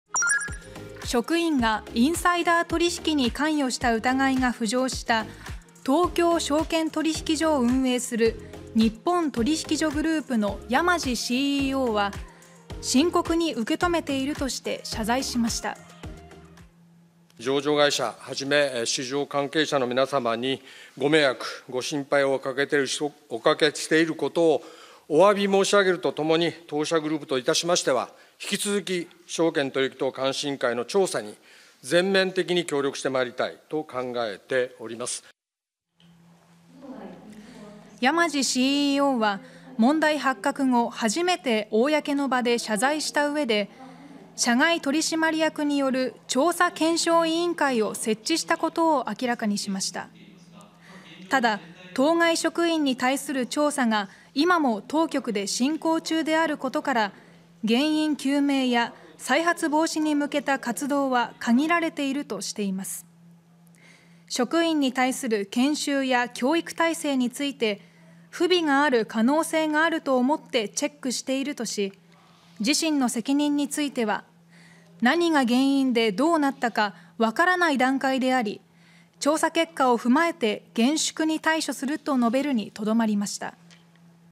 東証職員がインサイダー疑惑 日本取引所CEOが謝罪 調査検証委員会設置(2024年10月29日)
職員がインサイダー取引に関与した疑いが浮上した東京証券取引所を運営する日本取引所グループの山道CEO（最高経営責任者）は「深刻に受け止めている」として謝罪しました。

日本取引所グループ　山道裕己CEO
「上場会社はじめ、市場関係者の皆さまにご迷惑、ご心配をお掛けしていることをおわび申し上げるとともに、当社グループとしては引き続き、証券取引等監視委員会の調査に全面的に協力して参りたいと考えております」

　山道CEOは問題発覚後、初めて公の場で謝罪したうえで、社外取締役による調査検証委員会を設置したことを明らかにしました。

　ただ、当該職員に対する調査が今も当局で進行中であることから原因究明や再発防止に向けた活動は限られているとしています。

　職員に対する研修や教育体制について「不備がある可能性があると思ってチェックしている」とし、自身の責任については「何が原因でどうなったか分からない段階であり、調査結果を踏まえて厳粛に対処する」と述べるにとどまりました。